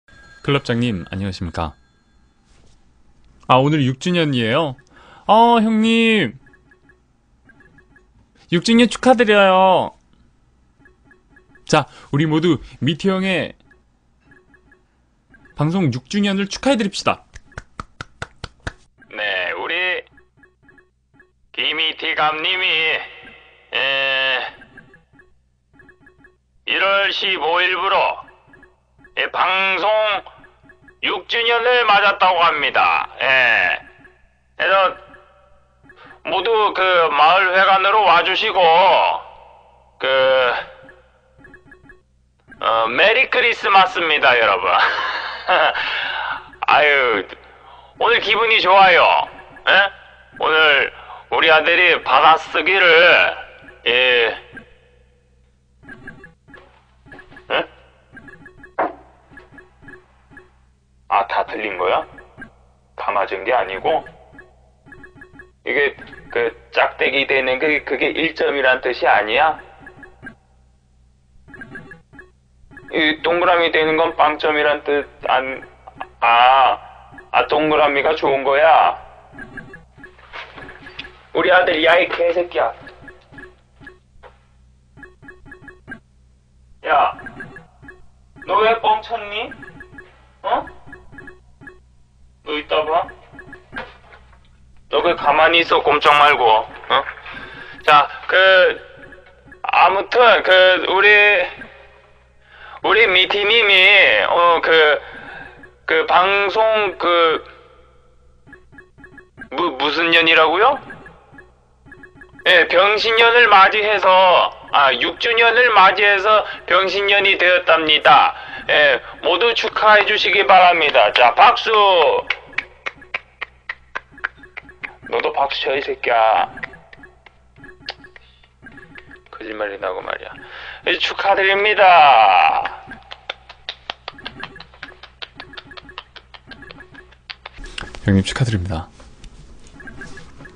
[유준호 영상] 축하 같지 않은 방송 6주년 축하 영상
미티형 방송 중에 갑자기 난입해서 축하해달라고 해주셔서 너무 감사드려요!
사랑해요!

인스타그램 / yuferesia